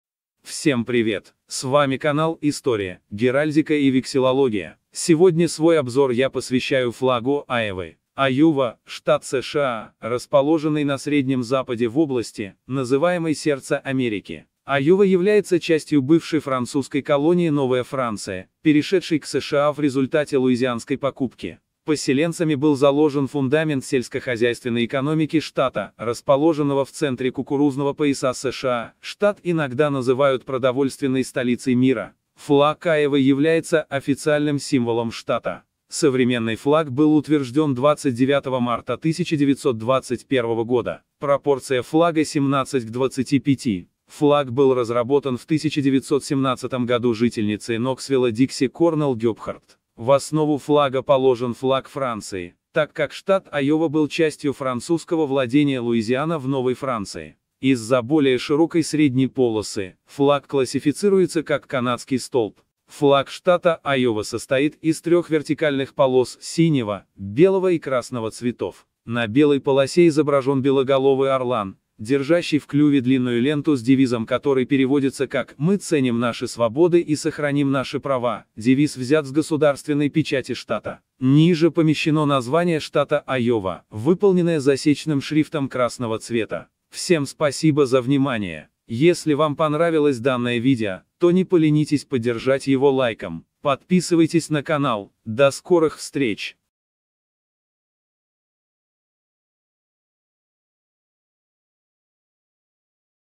Флаг Айовы. США.
Iowa flag. В этом видео Вы узнаете о флаге Айовы, его описание, историю и значение. И конечно же символизм элементов изображённых на нём.